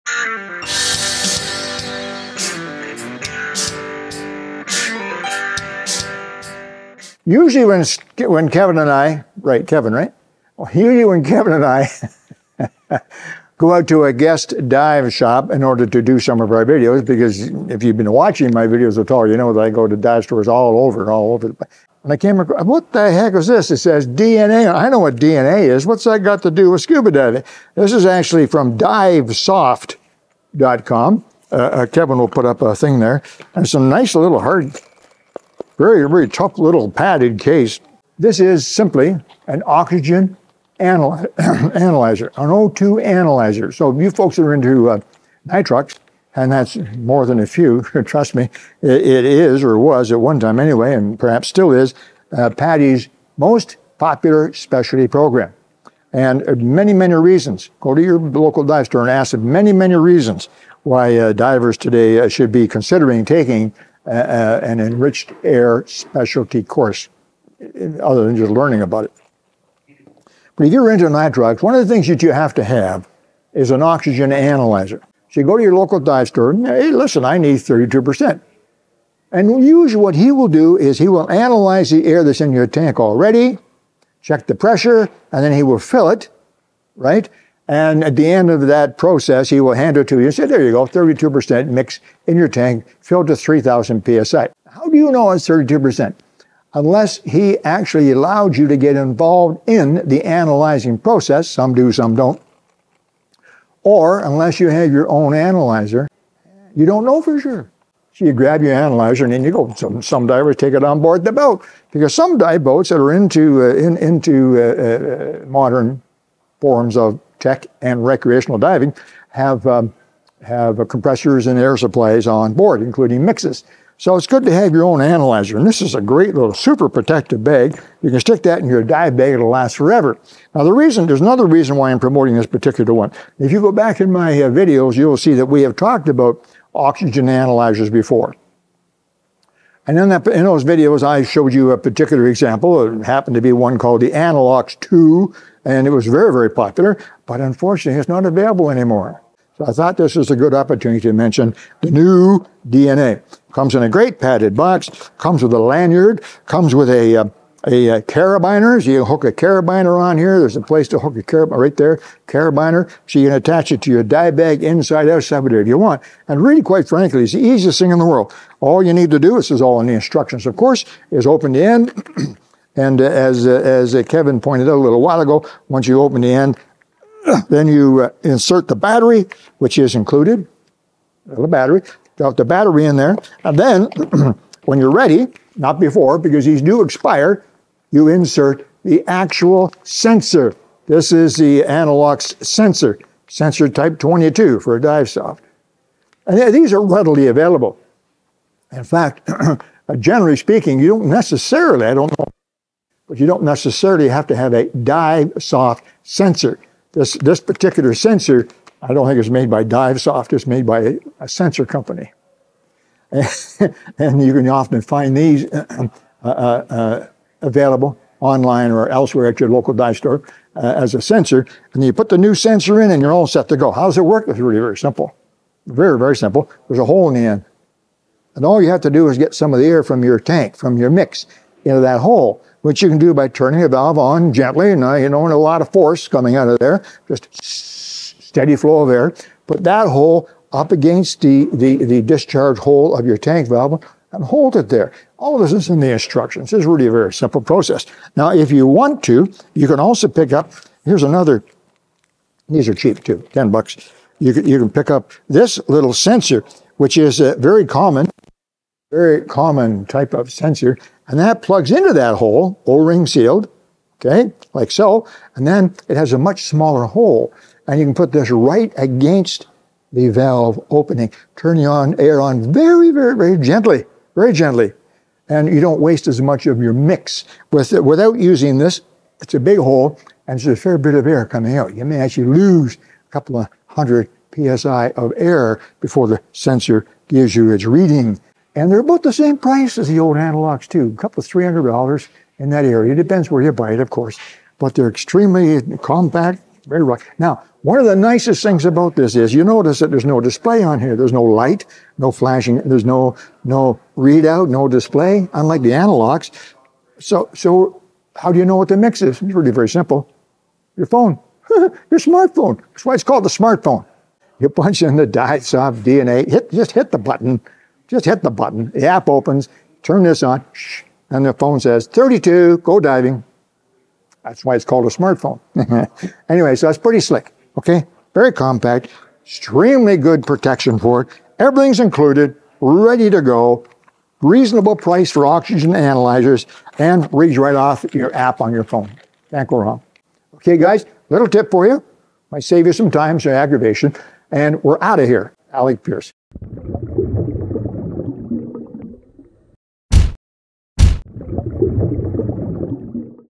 DNA Nitrox Analyzer | Scuba Tech Tips: S18E07
Enriched Air (Nitrox) certification is the most popular specialty so a good personal O2 analyzer is a good idea.  Alec reviews Divesoft's DNA Nitrox Analyzer for personal use with a smartphone.